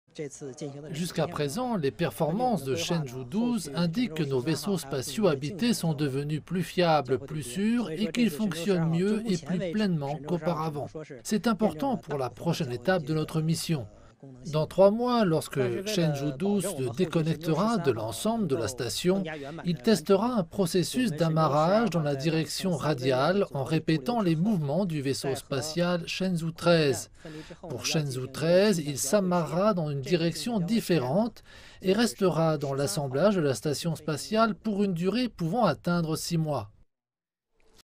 Arrimage réussi de Shenzhou-12 à Tianhe
Une étape historique a été franchie jeudi, lorsque le vaisseau spatial chinois Shenzhou-12 s'est amarré avec succès à Tianhe, le module central de la station spatiale chinoise. Mais plutôt que de se reposer sur leurs lauriers, les autorités spatiales annoncent se préparer à la prochaine mission.